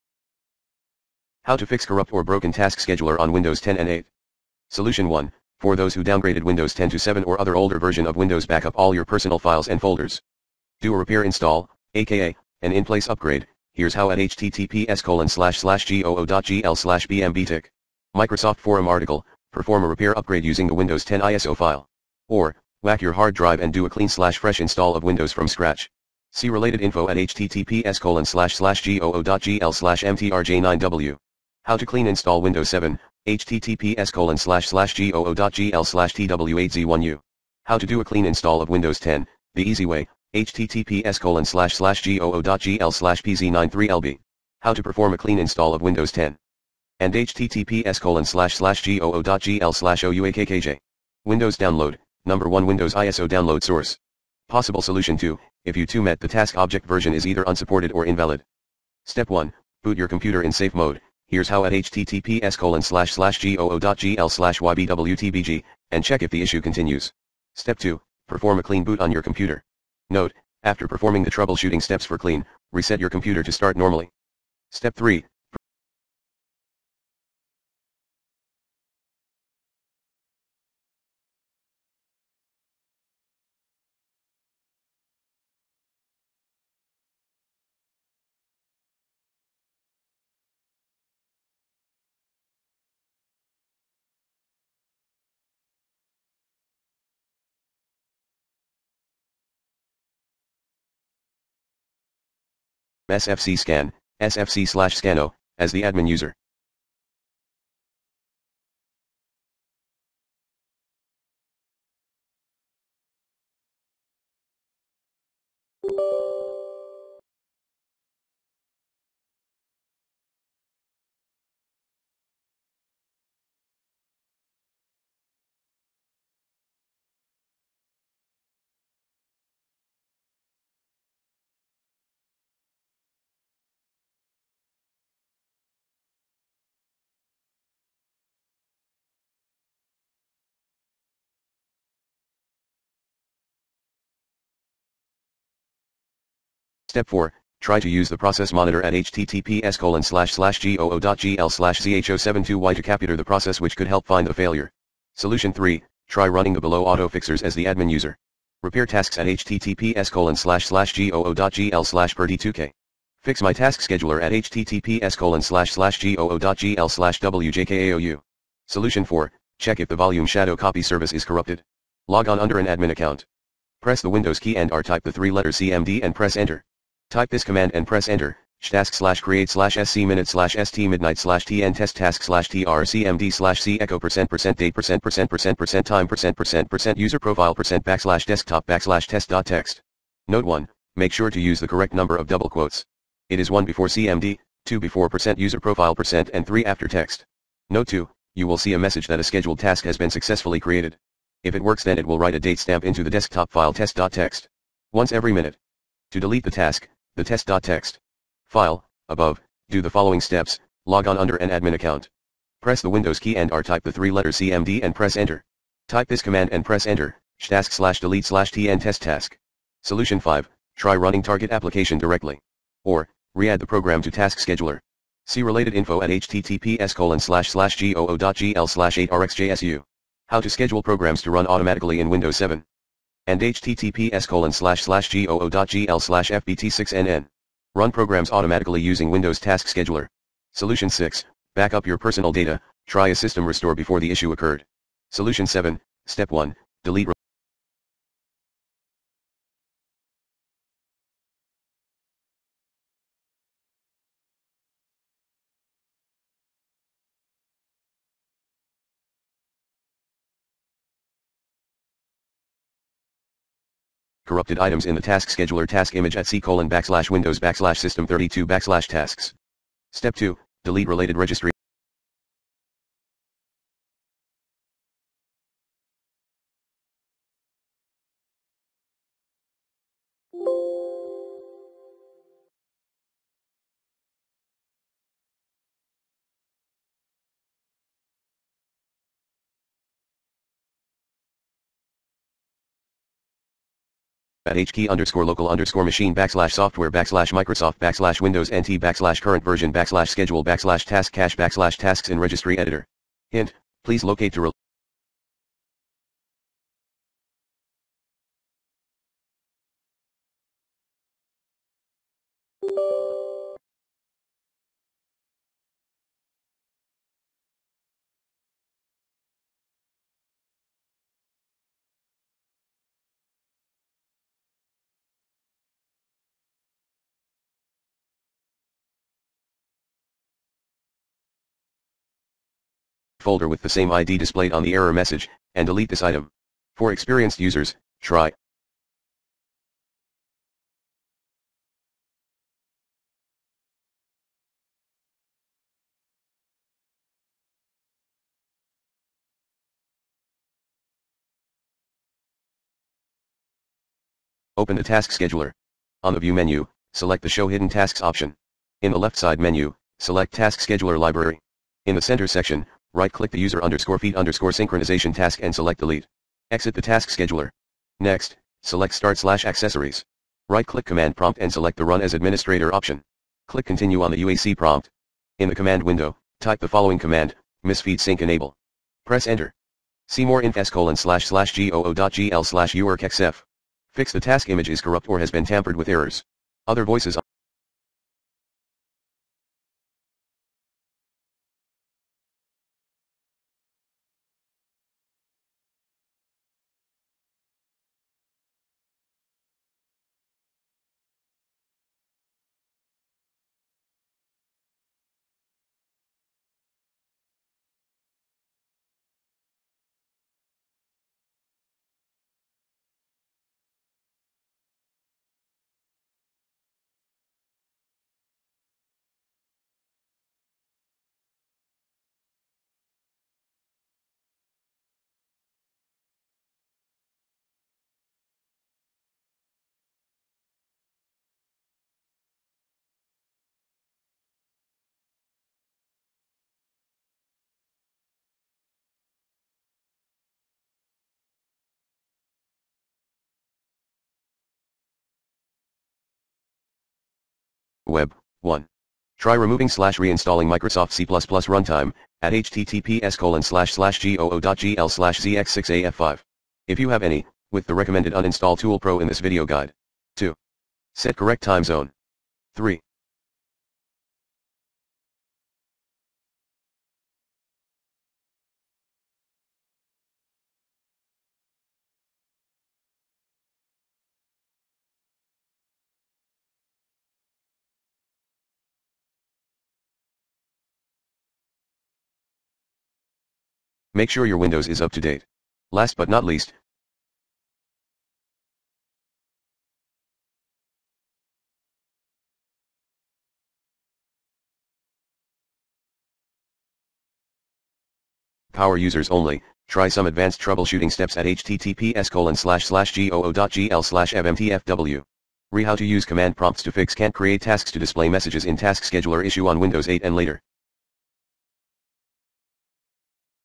Fix Corrupt/Broken Task Scheduler in Windows 10/8
This vid helps Fix Corrupt/Broken Task Scheduler problem on your Windows 10 or 8 computer.